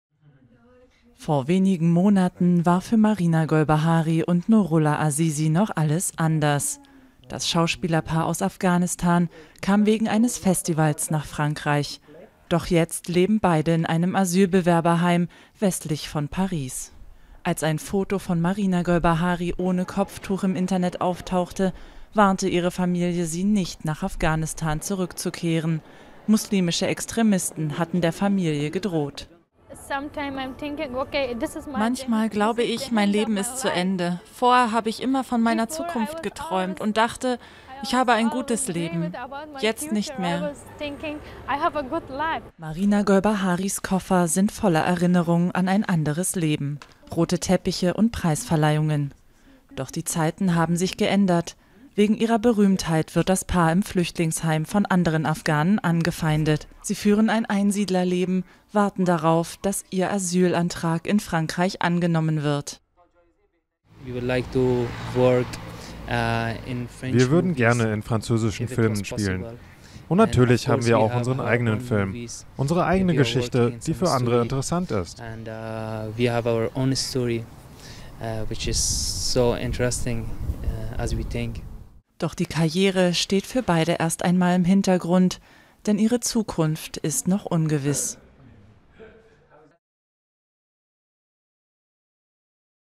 Filmstars in Afghanistan, Asylsuchende in Frankreich
Sie waren Filmstars in Afghanistan. Jetzt leben Marina Golbahari und Noorullah Asisi in einem heruntergekommenen Asylbewerberheim westlich von Paris. In ihrer Heimat ist ihr Leben durch religiöse Eiferer bedroht, denen Kino und selbstbewusste Frauen ein Dorn im Auge sind.